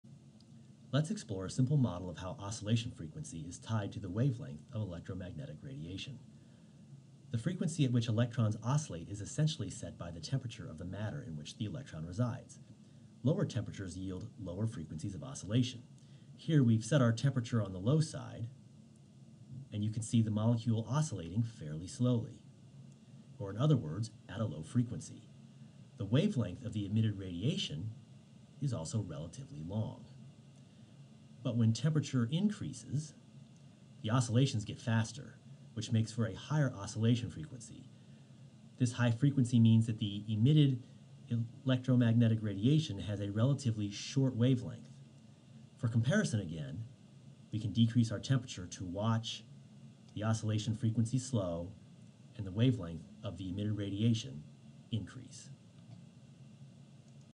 Temperature and EM Radiation Wavelength
A simplified model to demonstrate how oscillation frequency is tied to wavelength of electromagnetic radiation.

This video was created for Penn State's Meteo 3 course with the assistance of Steve Seman and the John A. Dutton e-Education Institute